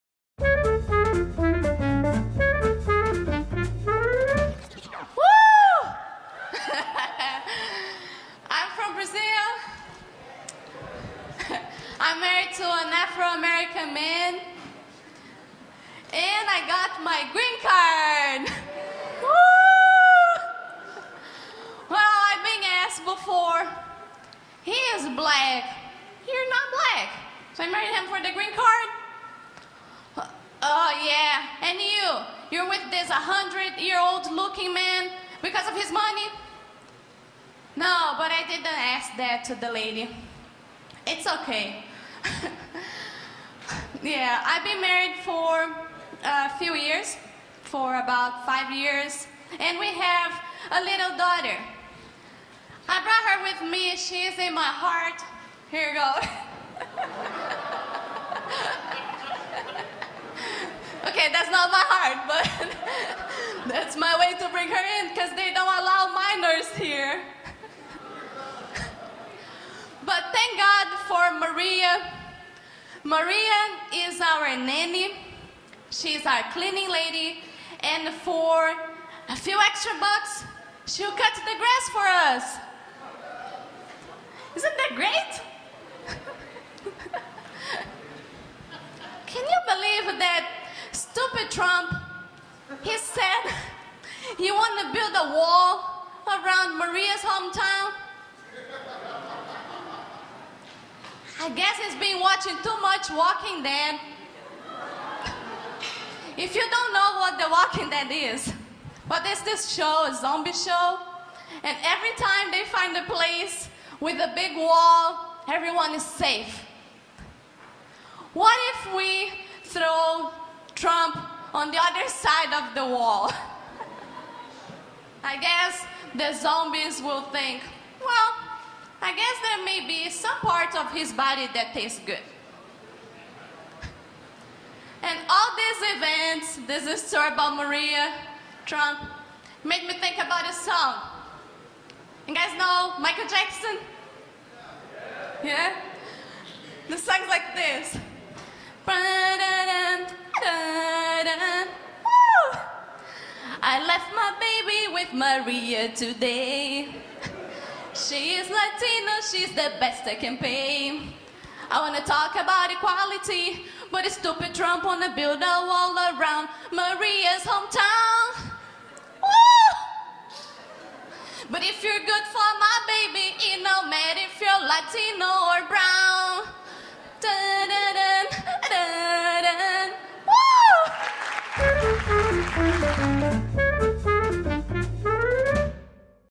Pri Thomas - 2016 Latin American Comedy Festival
Pri Thomas
2016 Latin American Comedy Festival
Flamingo Theater
Miami, FL